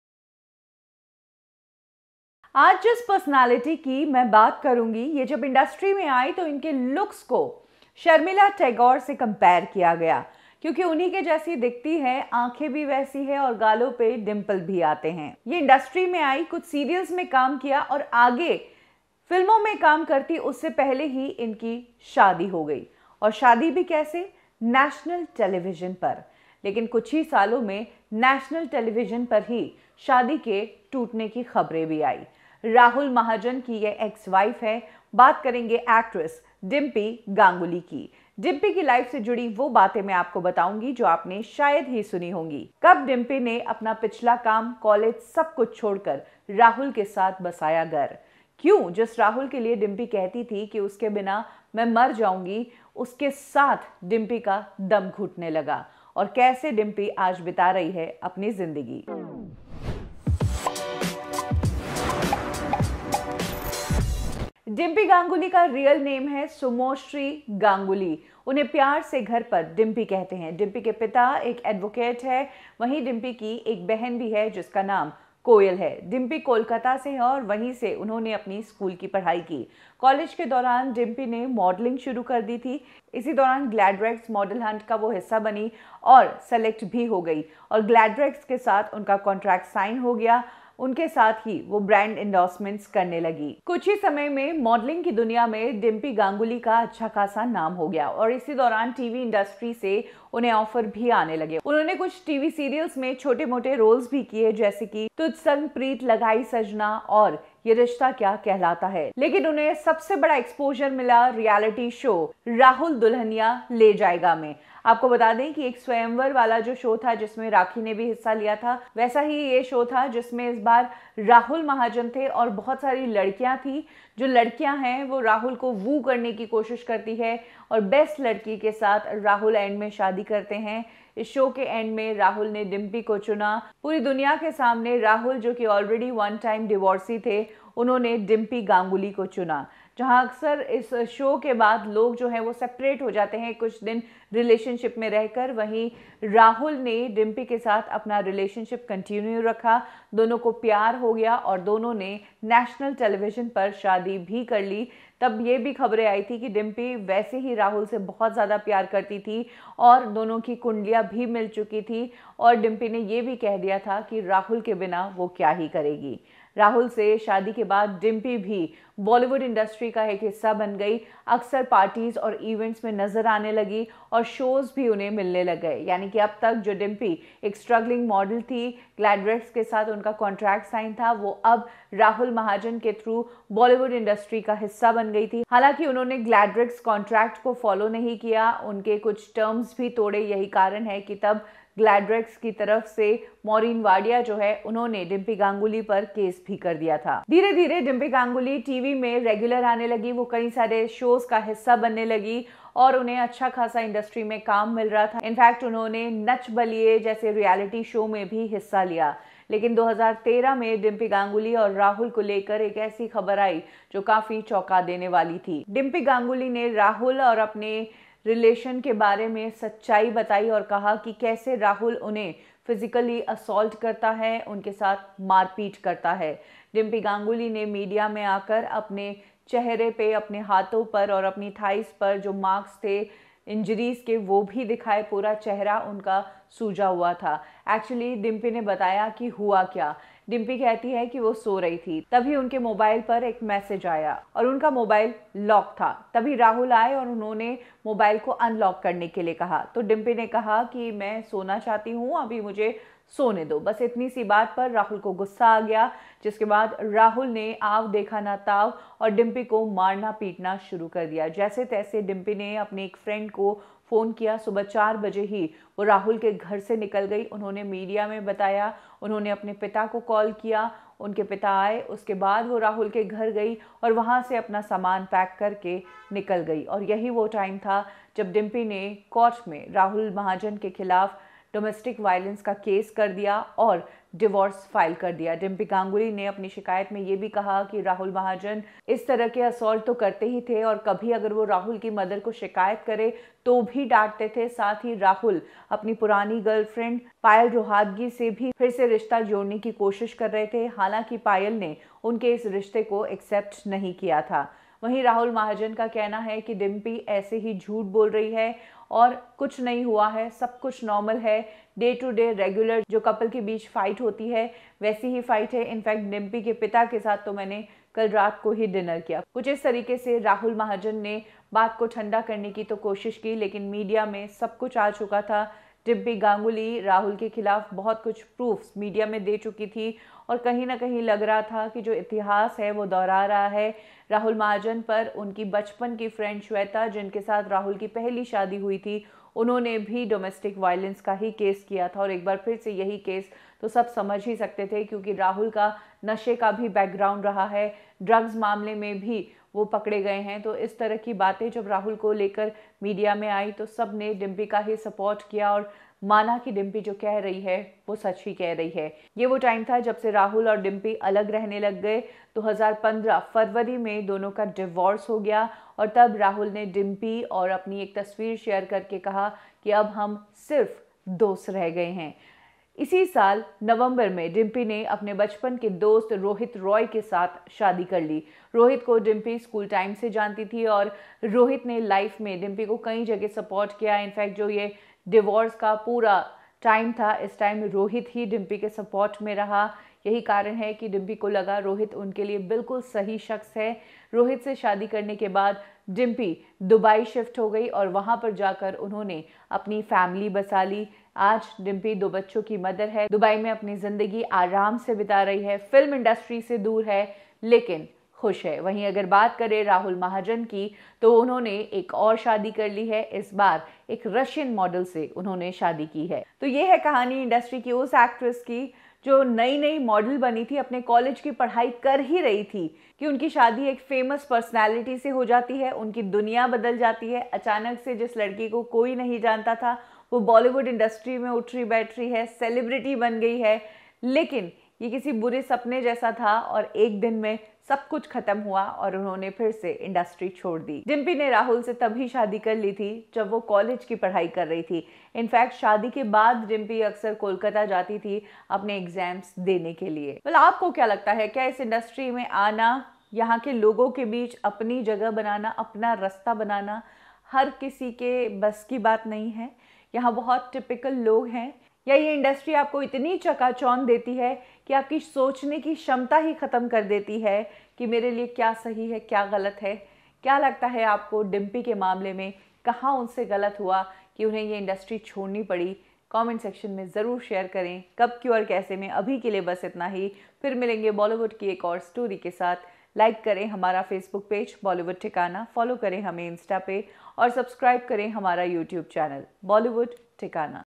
Dimpy Ganguli | कैसे Rahul Mahajan बन जाता था हैवान | कैसे फ़ोन पे आये एक Msg से हुआ दोनों का तलाक़
Dimpy Ganguli | Rahul Mahajan की EX Wife | कैसे फ़ोन पे आये एक Msg की वजह से हुआ दोनों का तलाक़